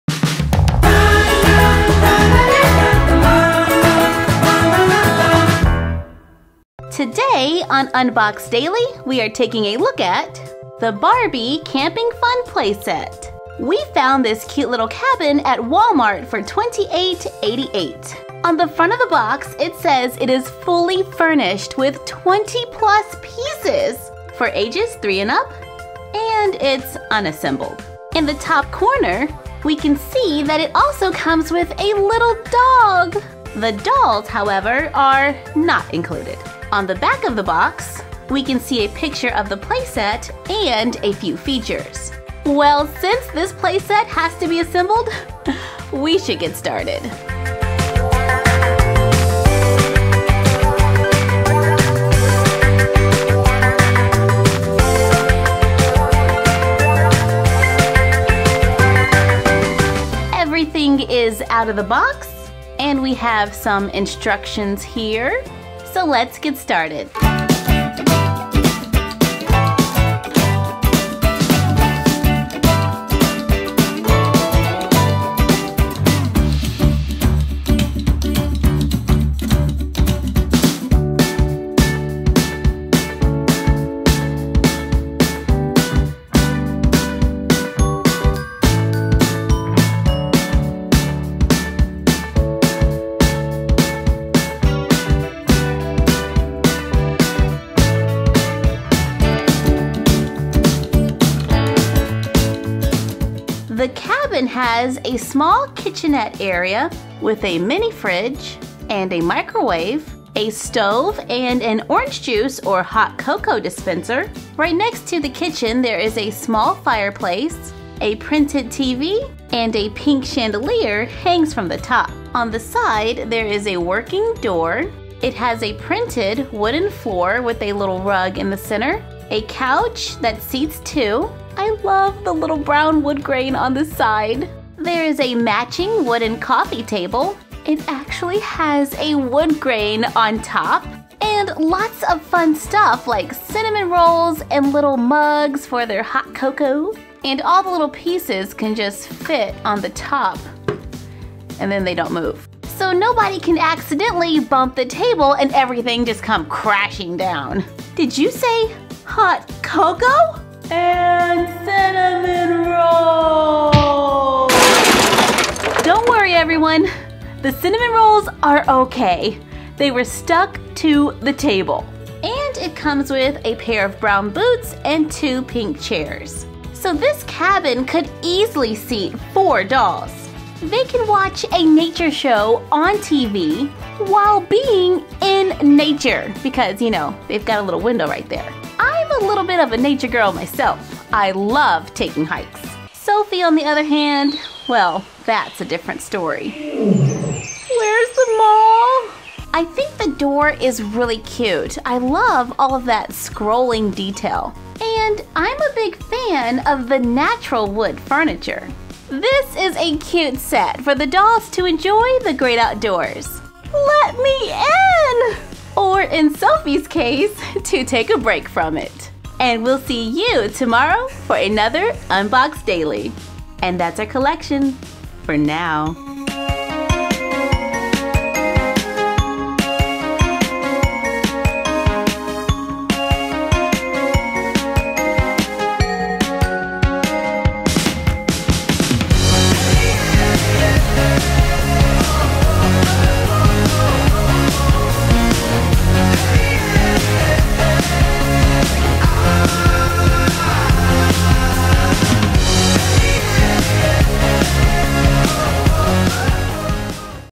Barbie Camping Fun Play set - Toy Review - 4K
by request: Today on Unbox Daily we review the Barbie Camping Fun Play Set!